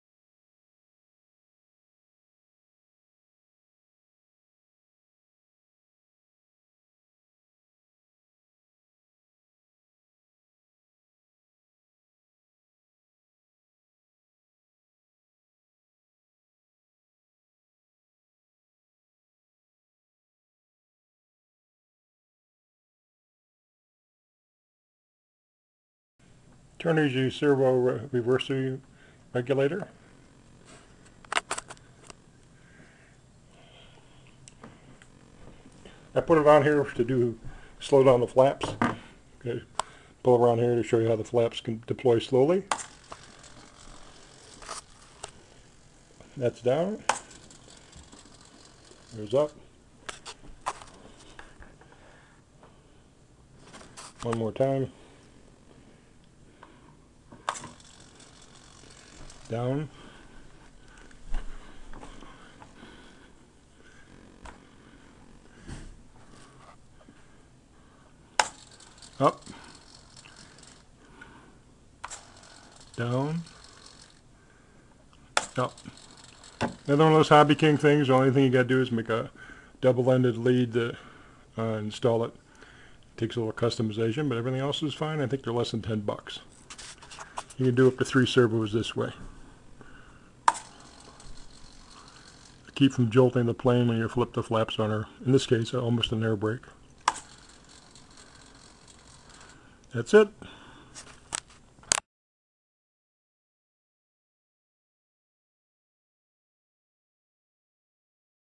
Turnigy 3 channel Adj. Slow Down Regulator -- Slowing Flap Deployment on the Skysurfer
Installation of Turnigy adjustable slow-down servo speed regulator -- 3 channel